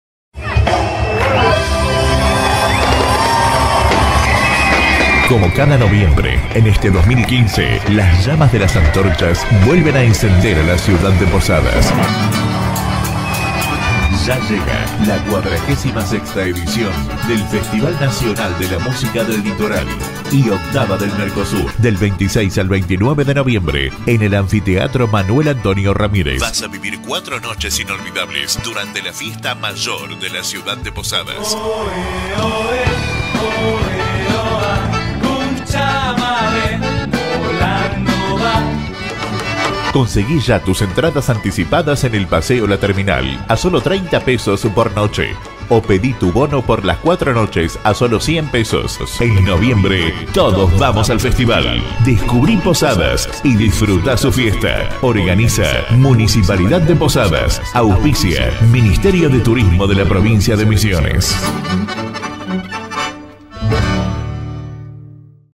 Spot Festival del Litoral 2015
Spot publicitario Institucional de la 46º Edición del Festival de la Música del Litoral y 8º del Mercosur.
Del 26 al 29 de Noviembre de 2015, en Posadas, Misiones, República Argentina, las antorchas del Festival del Litoral vuelven a encenderse. Descubrí Posadas y Disfrutá su Fiesta!